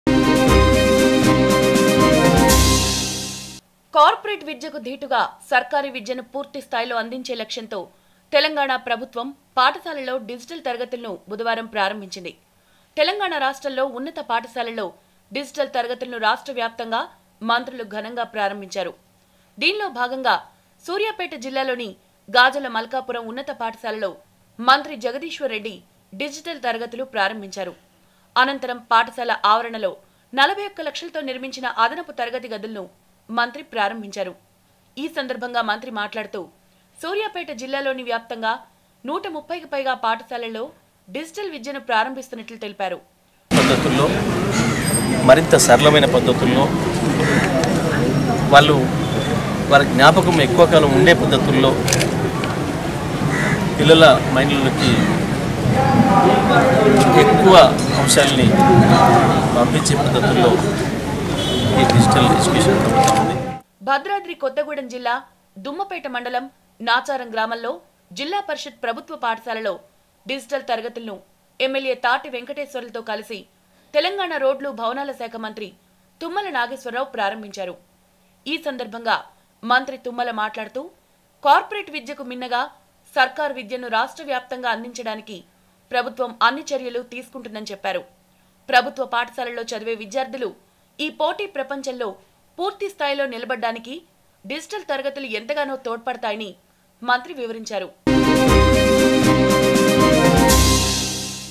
Telangana starts digital classes
హైదరాబాద్: కార్పోరేట్ విద్యకు ధీటుగా సర్కారీ విద్యను పూర్తి స్థాయిలో అందించే లక్ష్యంతో తెలంగాణ ప్రభుత్వం ప్రభుత్వ పాఠశాలలో డిజిటల్ తరగతులను బుధవారం ప్రారంభించింది. తెలంగాణ రాష్ట్రంలో ఉన్నత పాఠశాలల్లో డిజటల్ తరగతులను రాష్ట్రవ్యాప్తంగా మంత్రులు ఘనంగా ప్రారంభించారు. దీనిలో భాగంగా సూర్యాపేట జిల్లాలోని గాజులమల్కాపురం ఉన్నత పాఠశాలలో మంత్రి జగదీశ్వర్ రెడ్డి డిజిటల్ తరగతులు ప్రారంభించారు మరోవైపు భద్రాది కొత్తగూడెం జిల్లా దుమ్మపేట మండలం నాచారం గ్రామంలో జిల్లా పరిషత్ ప్రభుత్వ పాఠశాలలో డిజిటల్ తరగతులను ఎమ్మెల్యే తాటి వెంకటేశ్వర్లుతో కలిసి తెలంగాణ రోడ్లు భవనాల శాఖ మంత్రి తుమ్ముల నాగేశ్వరరావు ప్రారంభించారు..